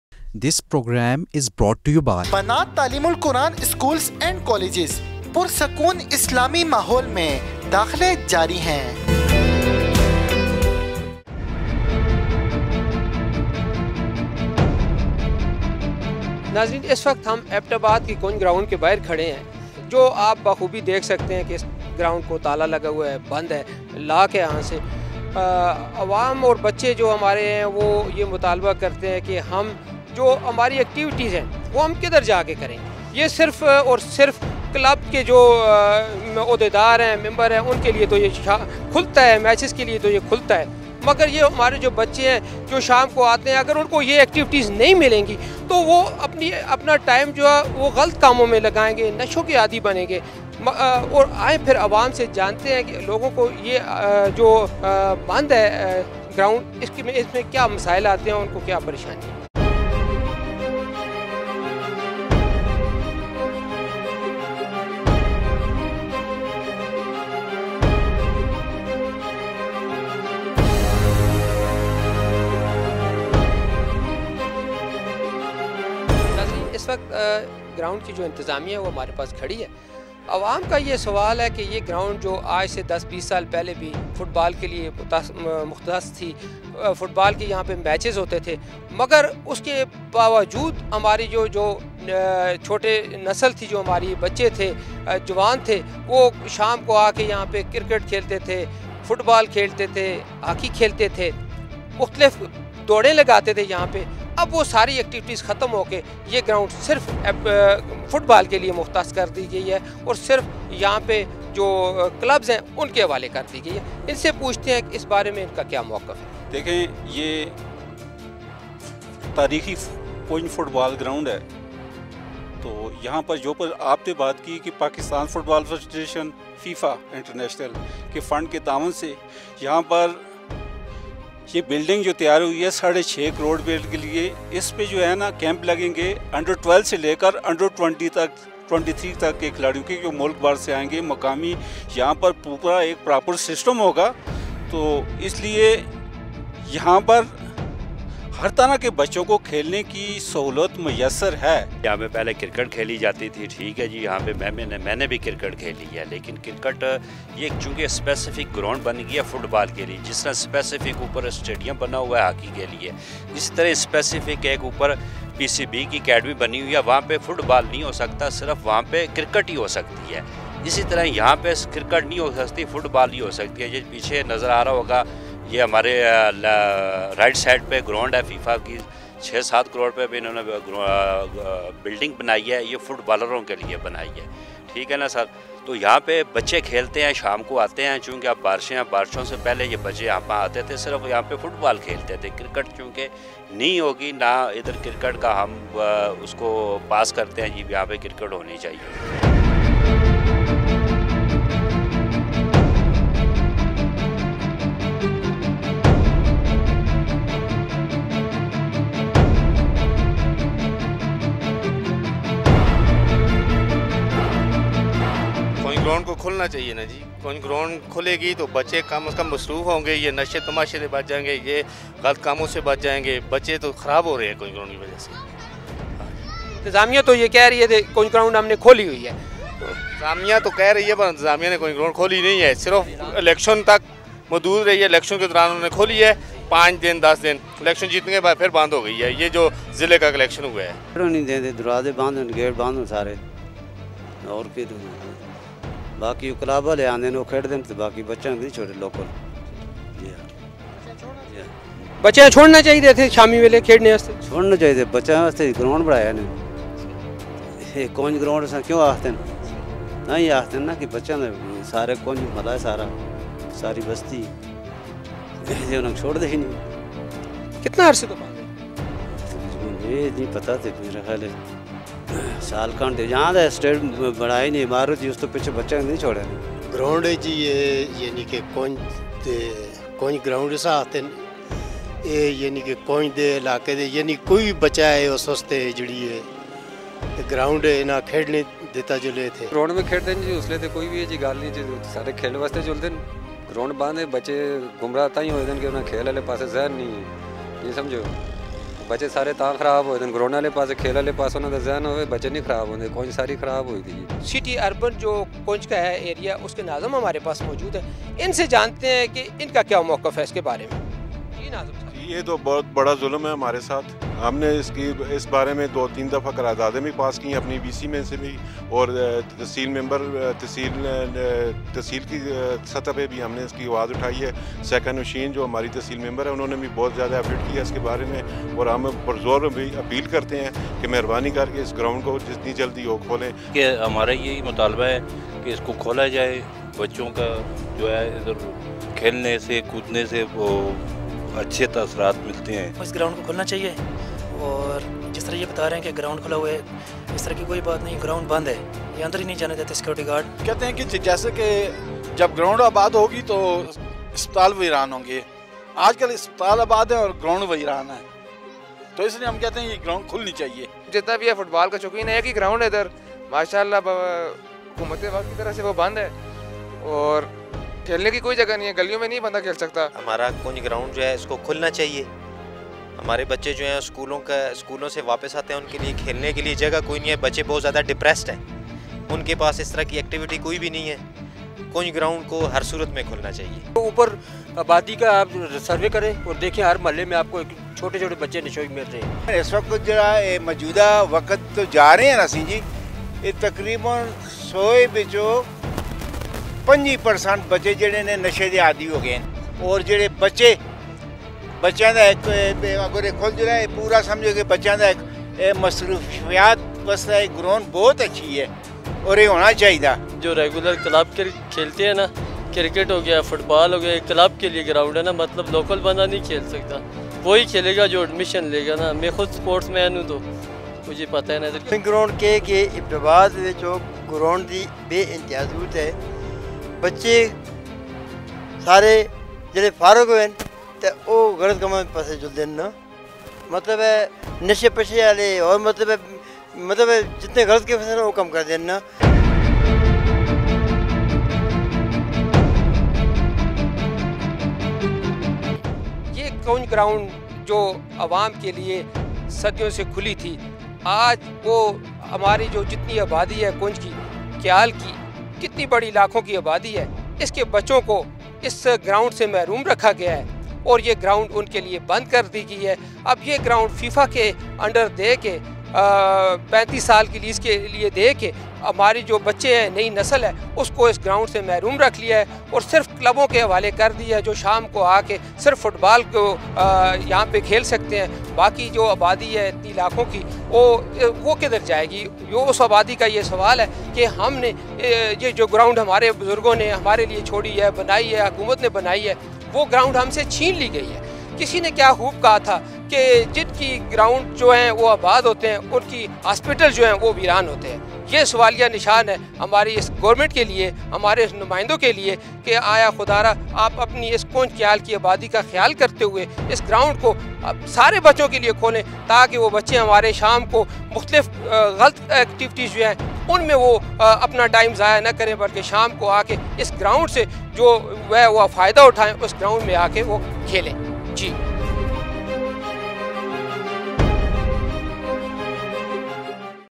Kunj Ground ISSUE Hazara1
ایبٹ آباد ضلع میں پہلے سے موجود بناۓ گیے گراونڈ بھی بچوں کے لیے آباد نہ کیے جا سکے۔کنج گراونڈ ایبٹ آباد بھی صرف کلب کے کھلاڑیوں تک محدود ہو گیا۔مزید اشفاق کیانی کی یہ رپورٹ دیکھیے